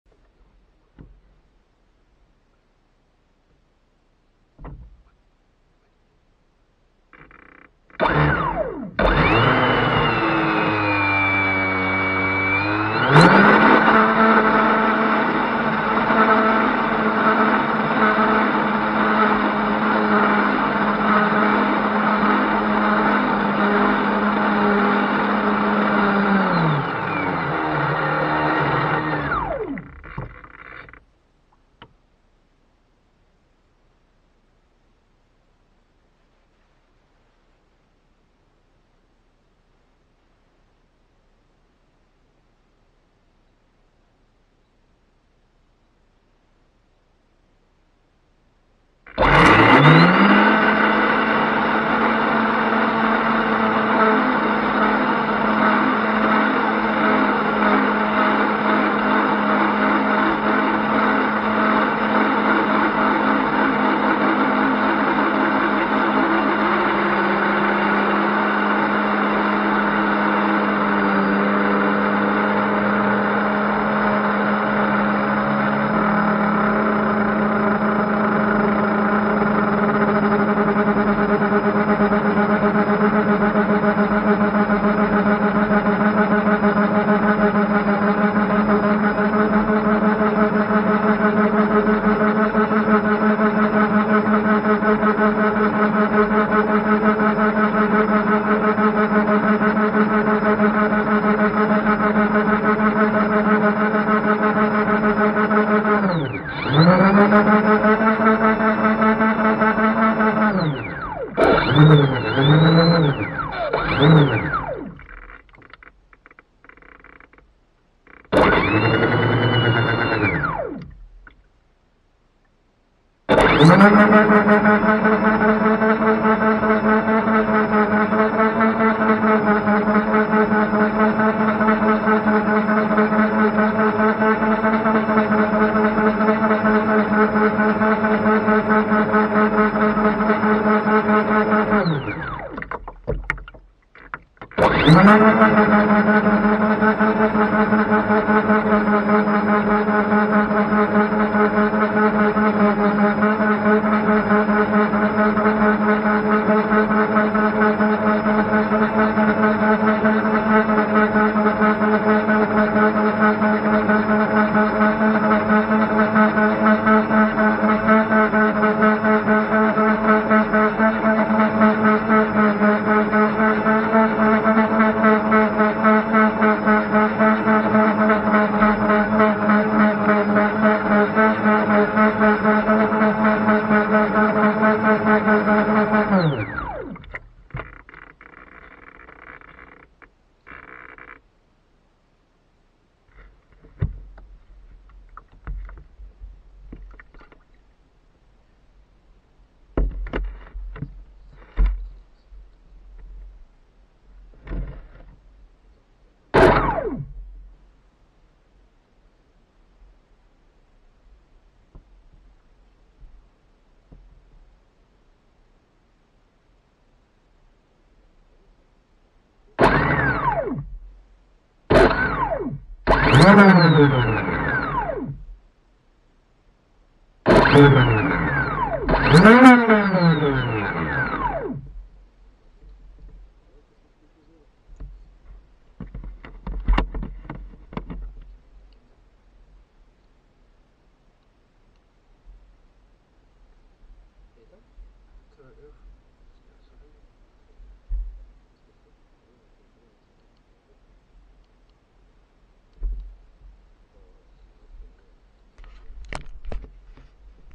akaso BIG STORM RC CAM ENBAR bateau catamaran
teste akaso elite en bateau rc josway big storm